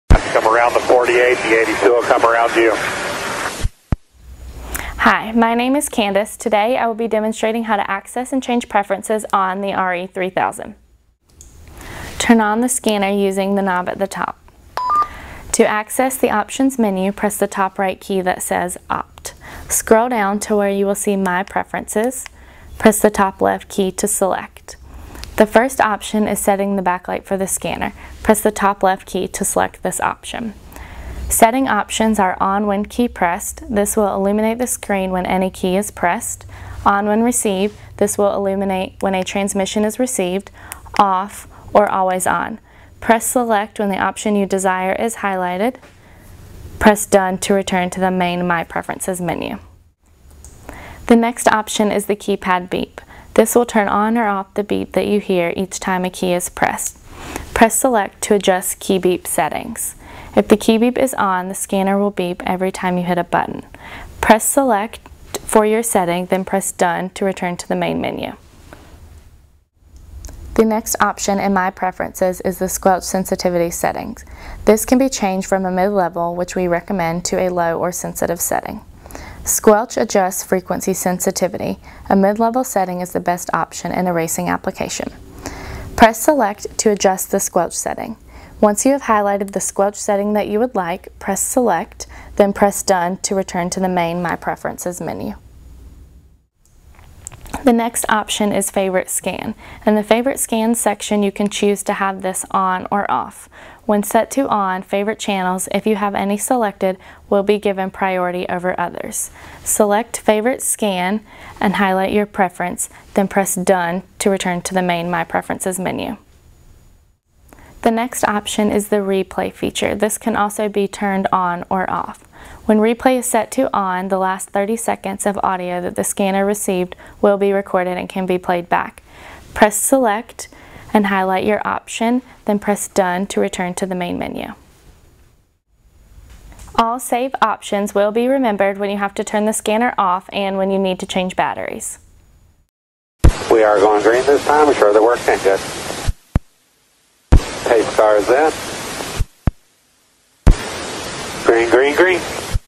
RE3000 Setting Your Scanner Preferences
nstructional video covering tall the setting in the "My Preferences" menu of the RE3000 scanner by RACING ELECTRONICS.